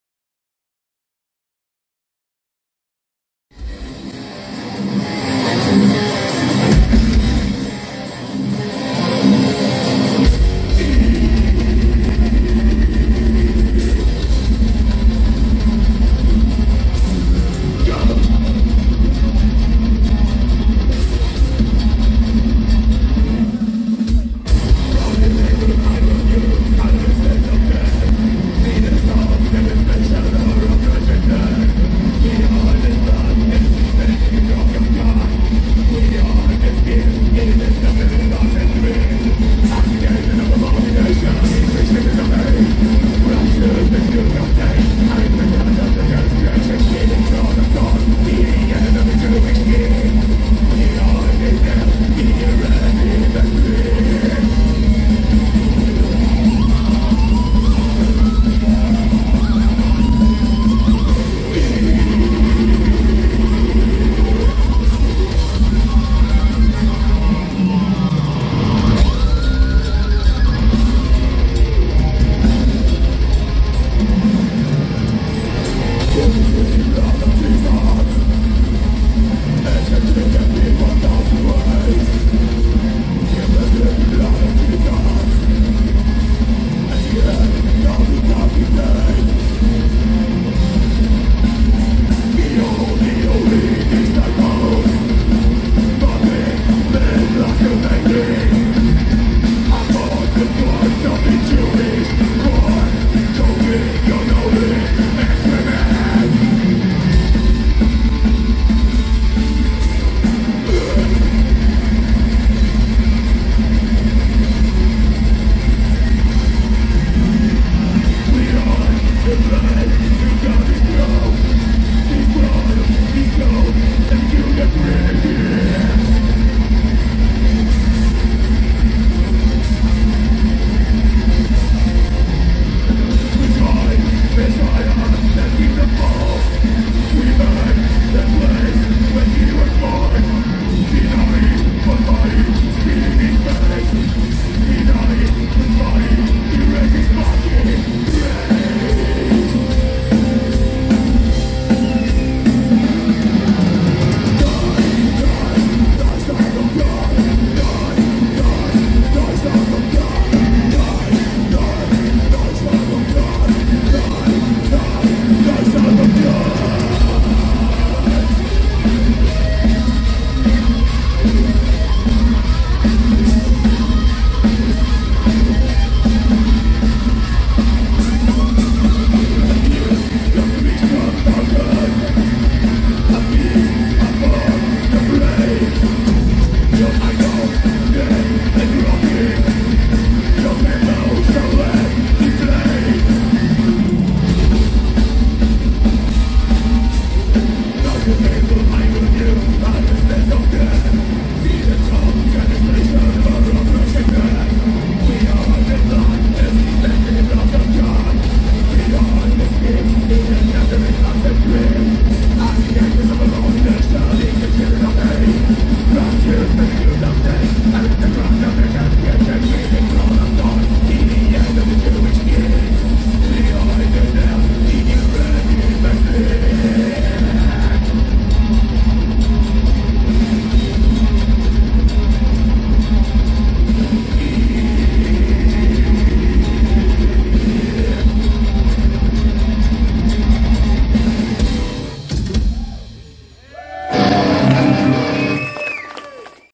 Infernal War Black Waves Fest Jarocin 09.10.2021 nr 1
Koncert zespołu Infernal War na Black Waves Fest w Jarocinie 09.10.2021